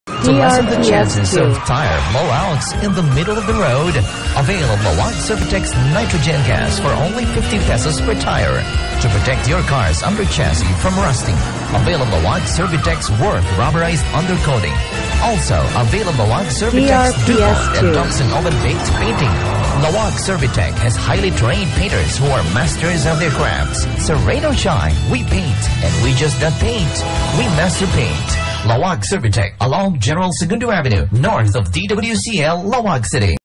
Laoag Servitek's Nitrogen Gas Service Radio Ad 2021-2022 33s (Laoag City)
This is the Provincial Radio Ad for Laoag Servitek in Laoag City, offering for their Nitrogen Gas Service.

DRPS Two 2350: Laoag Servitek's Nitrogen Gas Service Radio Ad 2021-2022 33s (Laoag City)